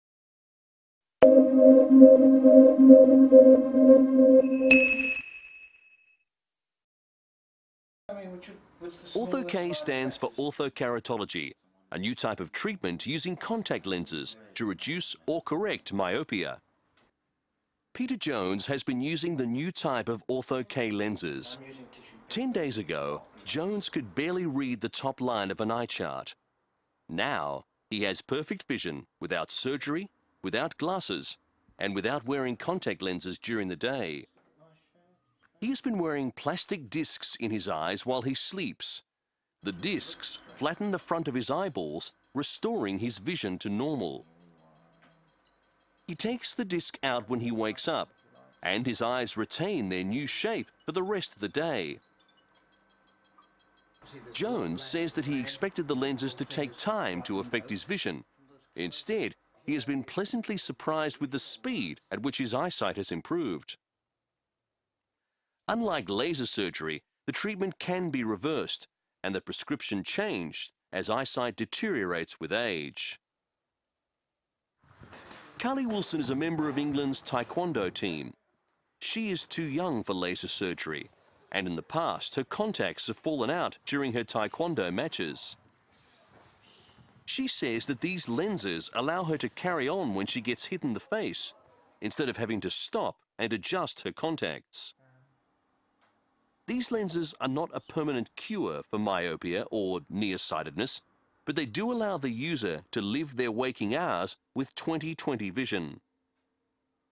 Ortho-K - Night Time Contact Lenses - INVENT [BroadbandTV]
Ortho-K stands for orthokertaology, a new type of treatment using contact lenses to reduce or correct myopia.